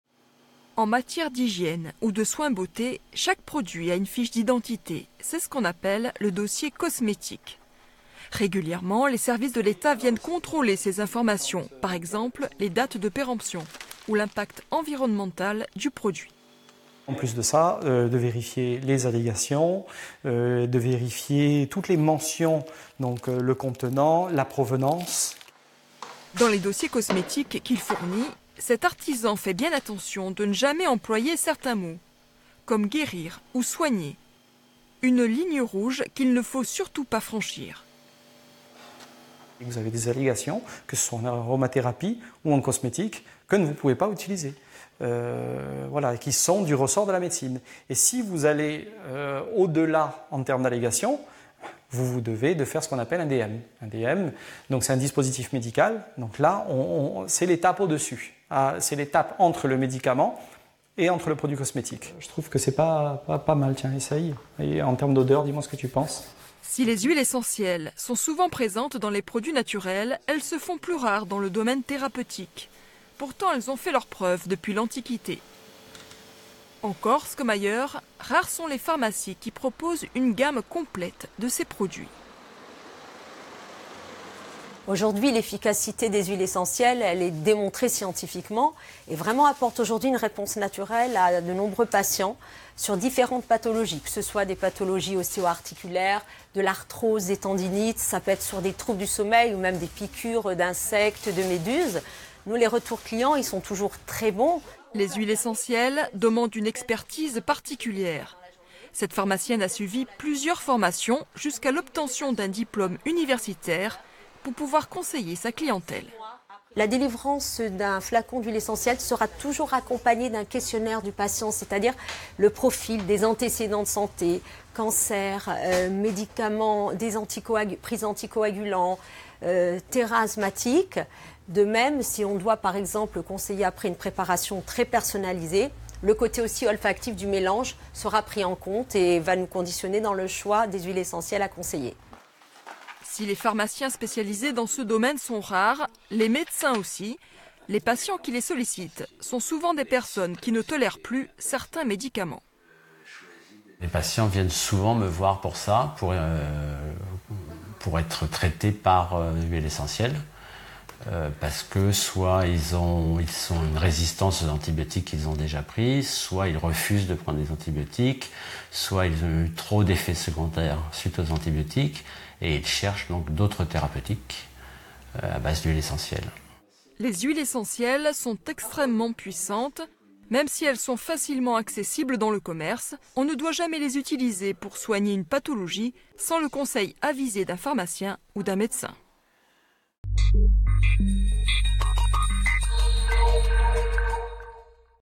Tema - Les huiles essentielles, volet 3
Les huiles essentielles sont utilisées de plus en plus dans le secteur de la cosmétique. Au niveau thérapeutique elles ont aussi fait leurs preuves depuis longtemps, pourtant, et c'est un paradoxe, elles sont encore très peu utilisées dans le milieu médical.

Intervenants - Christophe Fouilleron, gérant d'une société de cosmétiques; Sylvia Lévy, pharmacienne à Bastia; Jean-Marie Colin, médecin

Equipe - Solange Graziani, Marion Fiamma